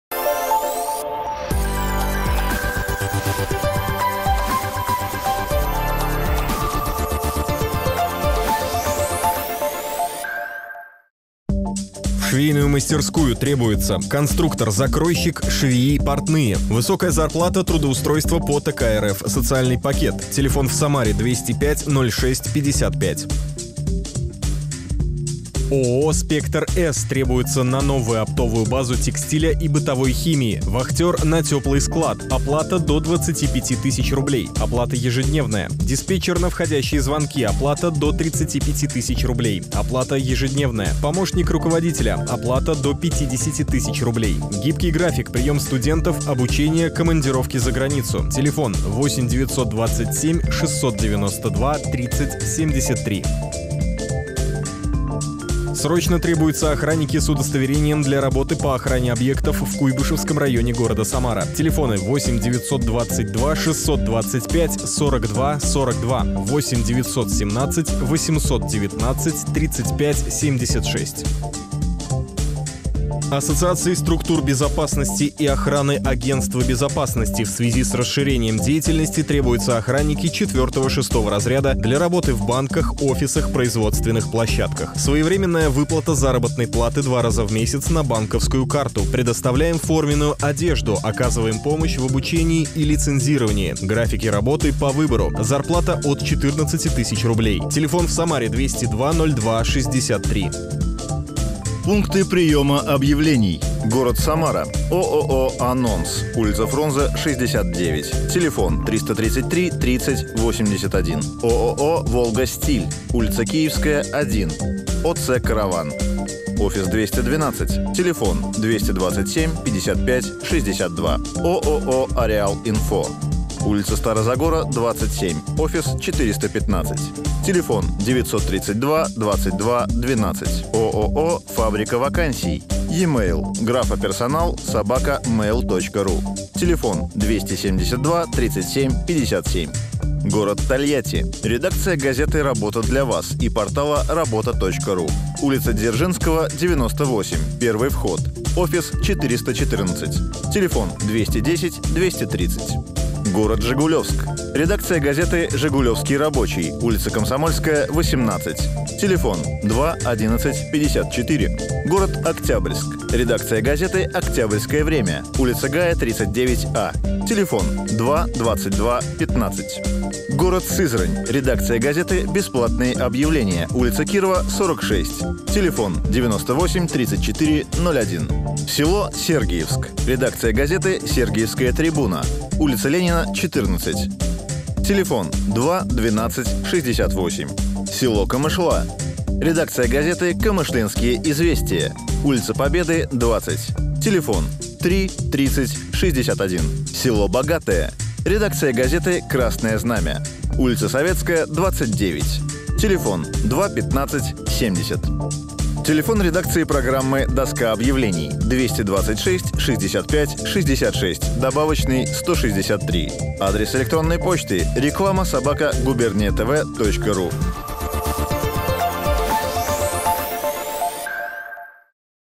Доска объявлений от 09.04.2018
В швейную мастерскую ТРЕБУЮТСЯ:
- Конструктор,
- закройщик,
- швеи,
- портные.
Высокая зп, трудоустройство по ТК РФ, соц.пакет

ООО «Спектр-С» требуются на новую опт. базу текстиля и быт. химии:
-Вахтёр на тёплый склад: оплата до 25 т. р.
Оплата ежедн-я
-Диспетчер на вход. звонки: оплата до 35 т р.
Оплата ежедн-я
-Помощник рук-ля: оплата до 50 т. р.
Гибкий гр., примем студентов, обучение, за гран. командировки.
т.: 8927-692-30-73

Срочно требуются охранники с удостоверением для работы по охране объектов в Куйбышевском районе г. Самара.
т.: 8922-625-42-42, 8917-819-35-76

Ассоциации структур безопасности и охраны
«Агентство безопасности»
в связи с расширением деятельности требуются охранники 4-6 разряда для работы в банках, офисах, производственных площадках. Своевременная выплата заработной платы 2 раза в месяц на банковскую карту, предоставляем форменную одежду, оказываем помощь в обучении и лицензировании.
Графики работы по выбору. З/п от 14000 руб.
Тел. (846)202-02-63